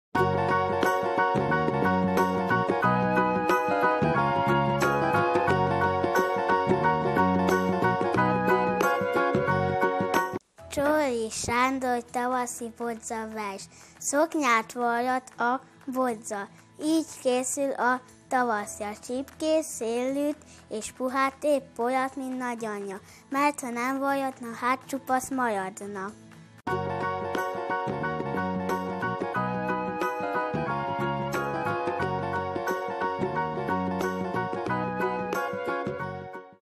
Kalamáris – Becskei Jasmina
Becskei Jasmina, Majsai úti Általános Iskola, Szabadka
(Csoóri Sándor: Tavaszi bodzavers)